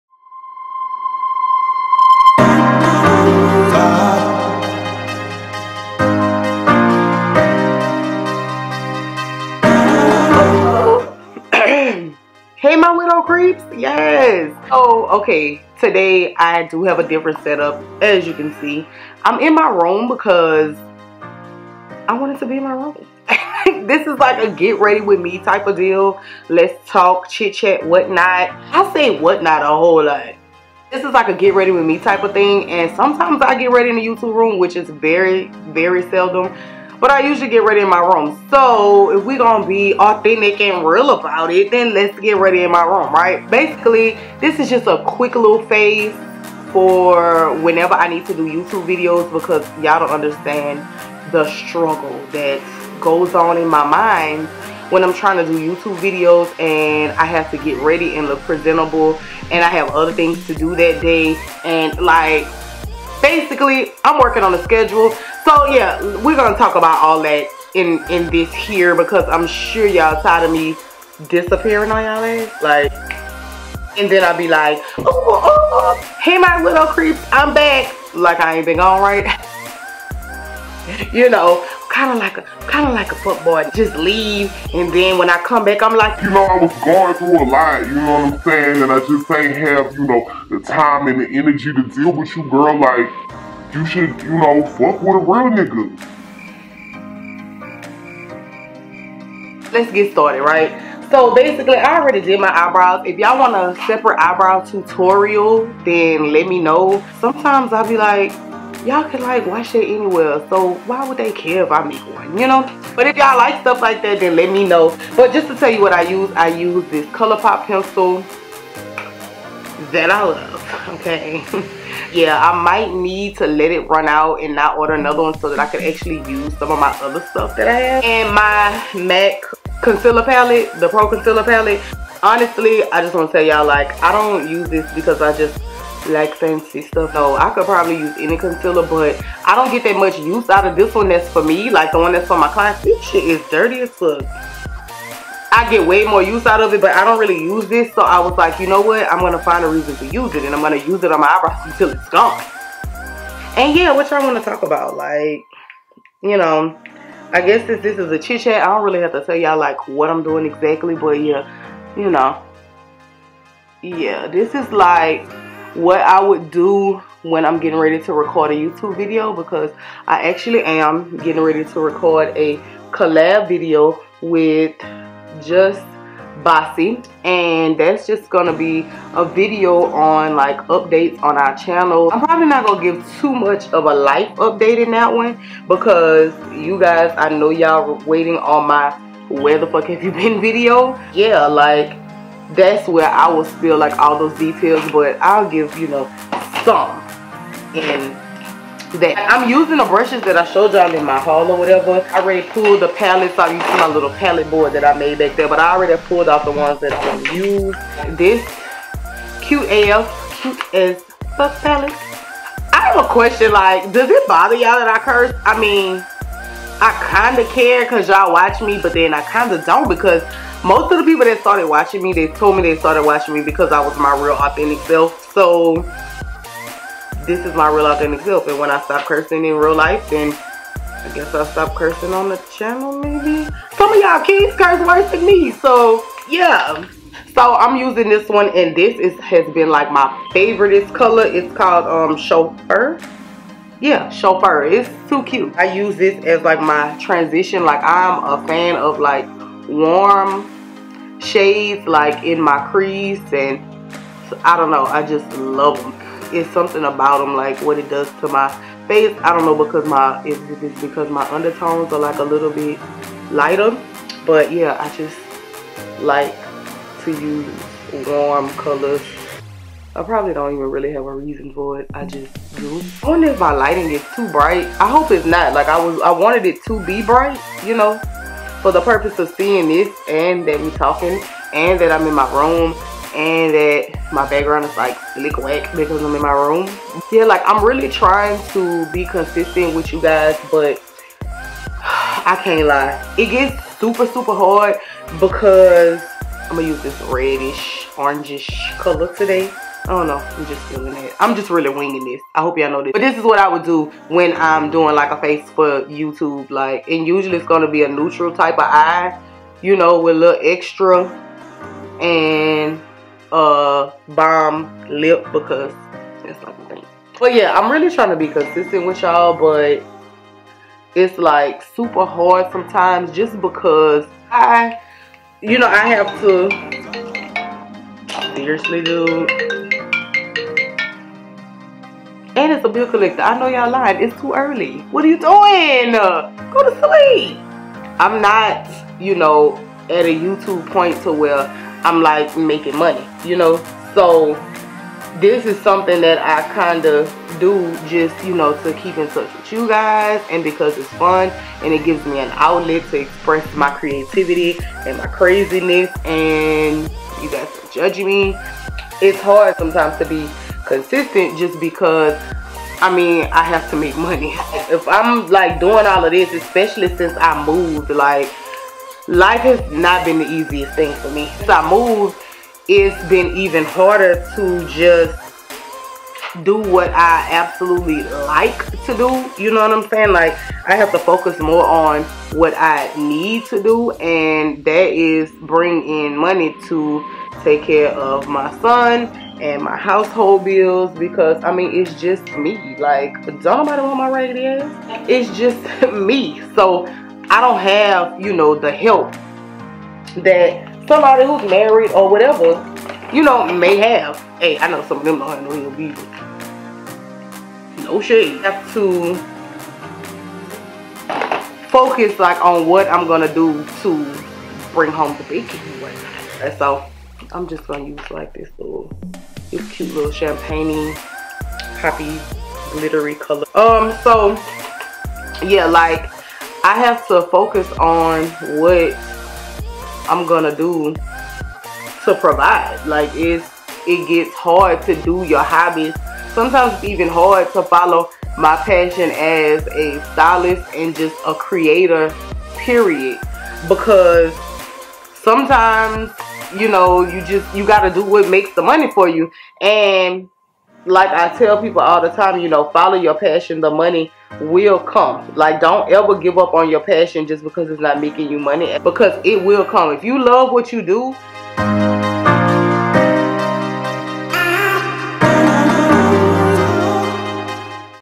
GRWM| 4 YouTube Recording Day & A Real Life Chit Chat fulla TEA! Part 1
this video is part 1 to my grwm featuring me just spilling tea about my life , sharing experiences and inspiration and just chatting it up while getting ready to get cute to film videos for the day ! so i hope u enjoy stay tuned for part 2!! love u kreeps !

Stalk ME I like it !!